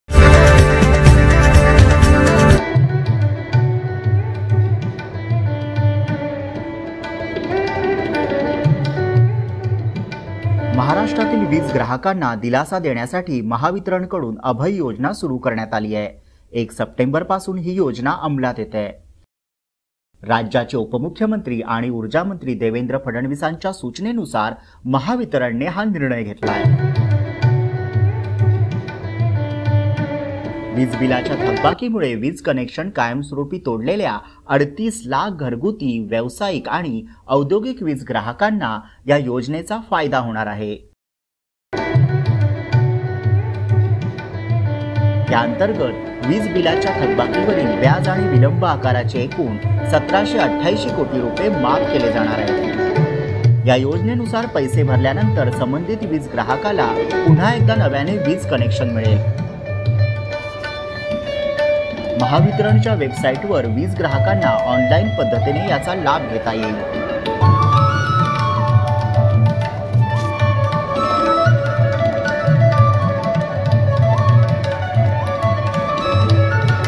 महाराष्ट्र के 38 लाख वीजली ग्राहकों को अभय योजना का लाभ
महाराष्ट्र सरकार के उर्जामंत्री देवेंद्र फडणवीस का बड़ा फैसला राज्य के 38 लाख बिजली बिल के बकायादारों का अभय योजना के अंतर्गत व्याज होगा माफ।
जिसके कारण किसानों एवं औद्योगिक क्षेत्र के लोगों कक मिलेगी बड़ी राहत।